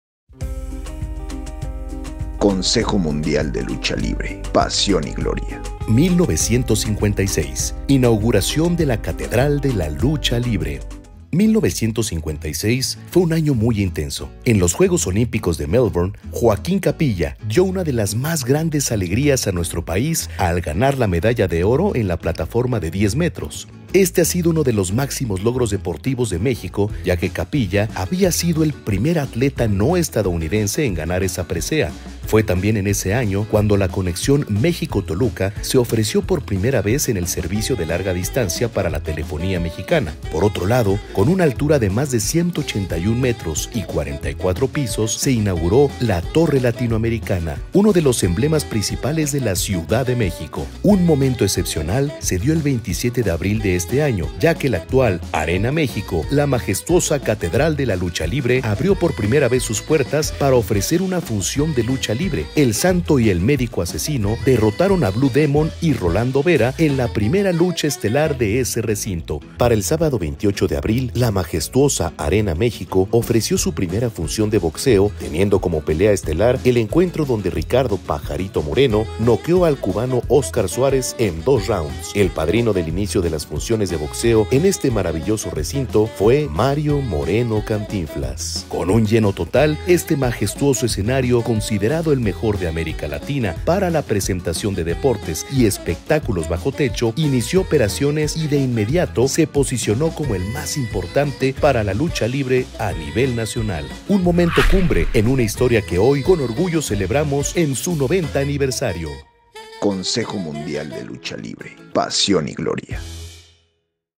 CMLL, PASIÓN Y GLORIA - 1956: INAUGURACIÓN DE LA CATEDRAL DE LA LUCHA LIBRE
En este año, la Arena México abrió sus puertas con una función de Lucha Libre y al día siguiente, con una de box.

Recuerda que faltan 67 días para la gran función del 90AniversarioCMLL.

📍Arena México
🗓️ Sábado 16 de Septiembre '23
🕔 5:00 p.m.